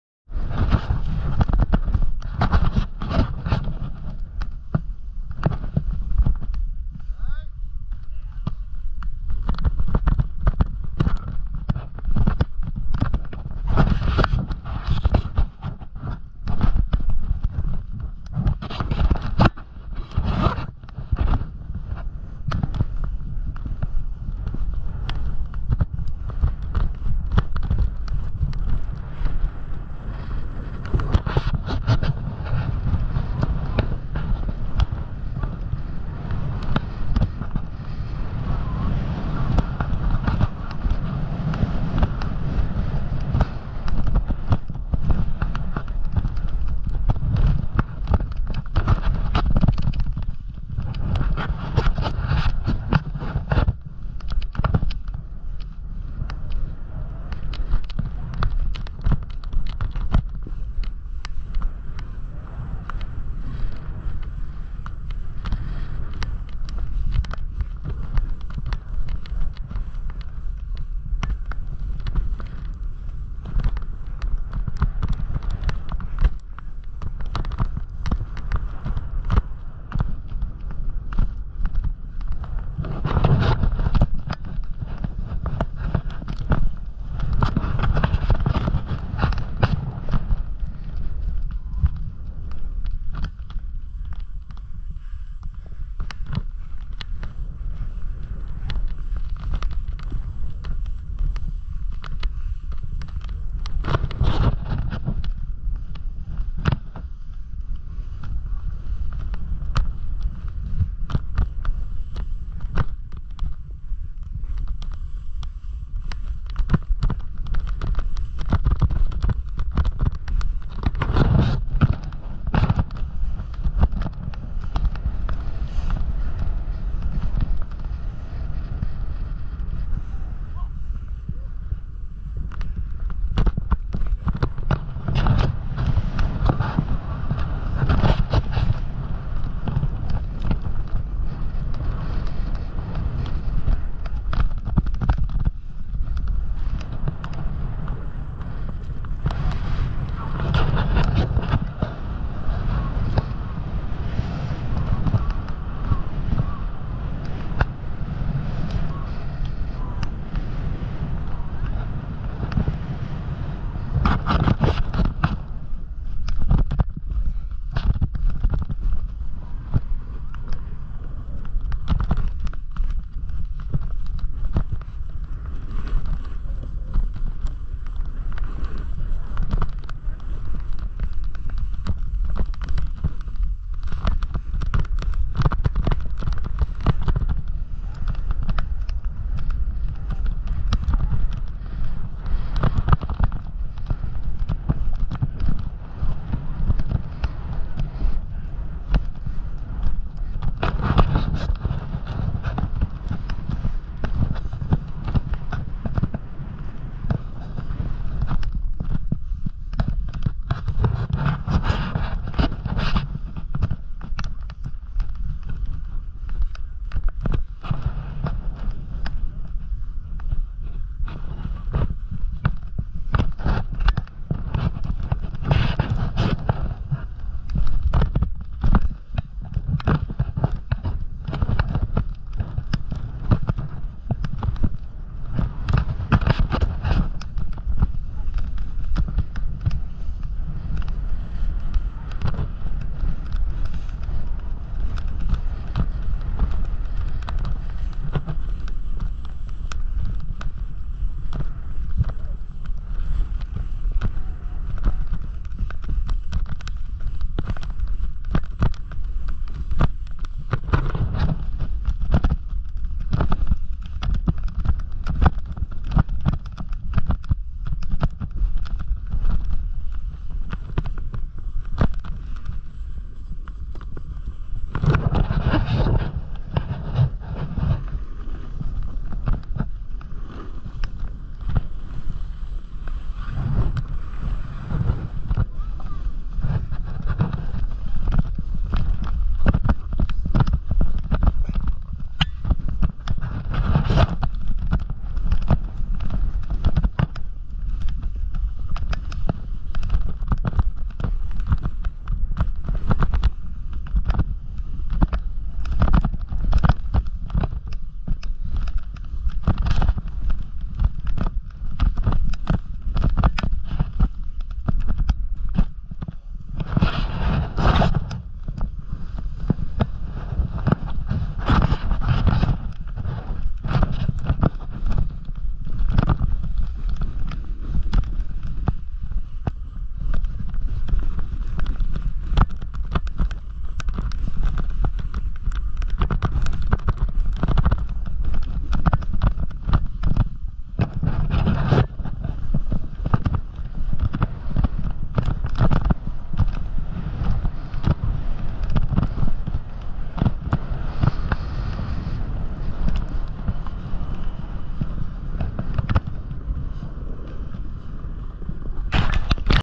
Sierra at Tahoe 1/6/2023
Powder day wooooooooo woooooo had so much fun @SierraAtTahoeResort